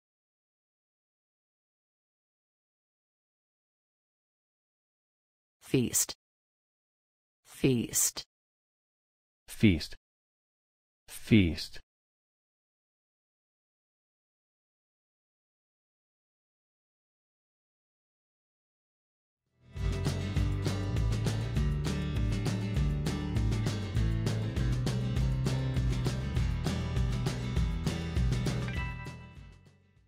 كيف أقول عيد باللّغة الإنجليزيّة
كيف أقول بشكل صحيح عيد باللّغة الإنجليزيّة؟
ما هي التّرجمة الصّحيحة لكلمة عيد من العربيّة إلى الإنجليزيّة؟
كيف تكتب بشكل صحيح كلمة feast?

شكرا جزيلا!